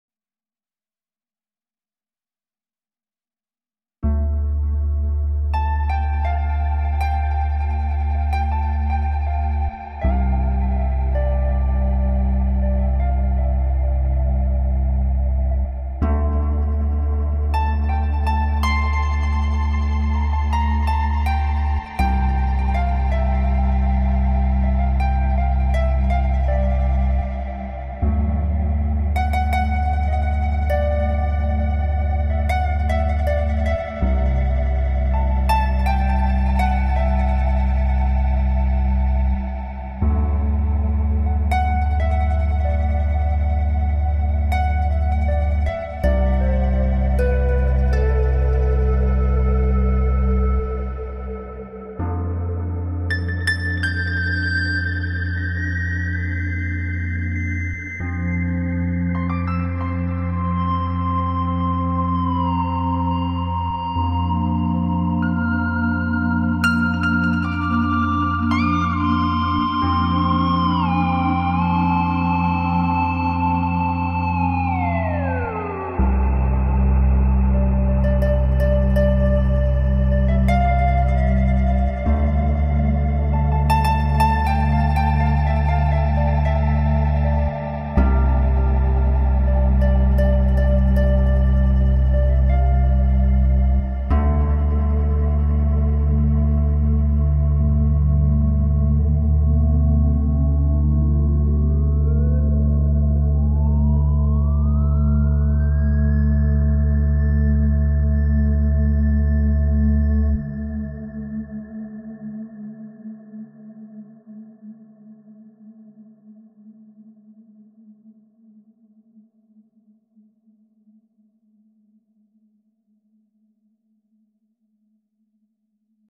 Luis Barrios - Roli Seaboard Rise 49 improvisation
Hey guys just wanted to share my AMAZING keyboard / the one and only ROLI SEABOARD RISE 49 - I absolutely loved it...it Rocks!!